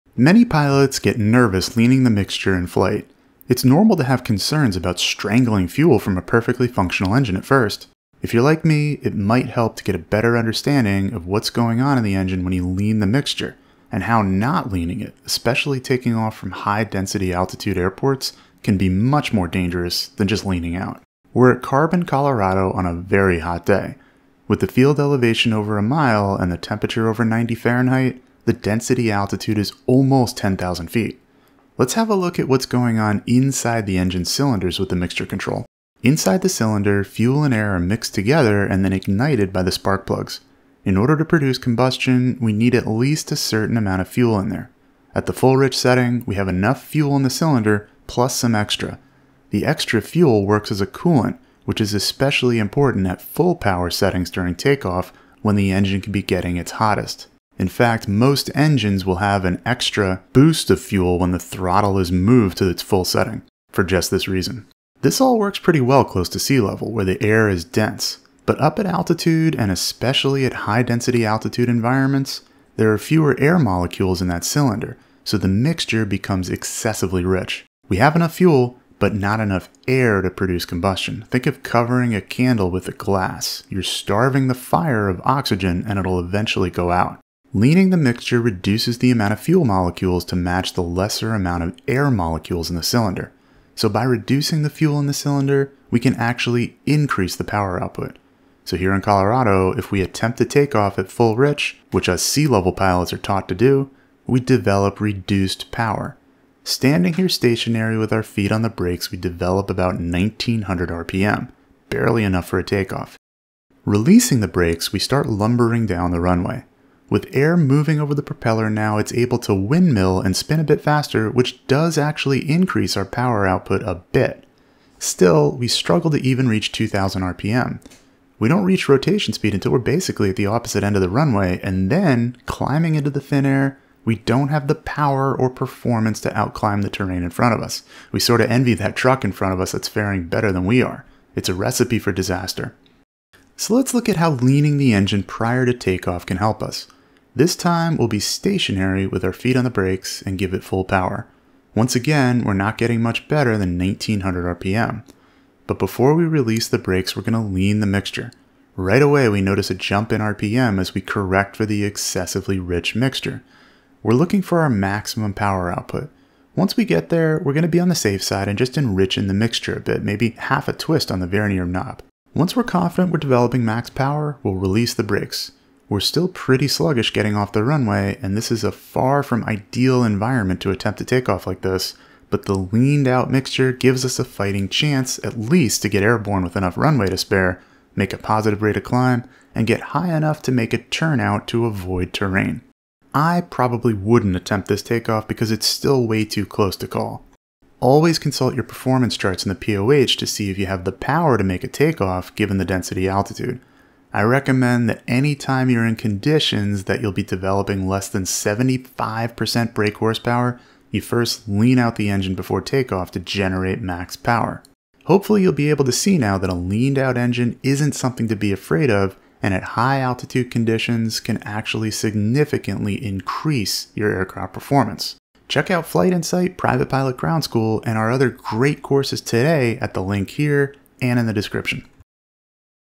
Why You Must Lean the Mixture at High Altitudes | Avoid This Dangerous Takeoff Mistake
Leaning the mixture before takeoff can feel counterintuitive—but at high-density altitude airports, not leaning can be downright dangerous. In this video, we break down what's really happening inside your engine when you adjust the mixture, and why taking off at full rich in places like Carbon, Colorado (DA near 10,000 feet!) can cripple your performance.

We’ll walk you through the physics inside the cylinder, show a real-world test of power output with and without leaning, and explain how to safely optimize your engine for max performance in high-altitude environments.

👉 Learn why leaning increases power at altitude
👉 Watch a side-by-side takeoff performance demo
👉 Understand how to apply mixture leaning safely and confidently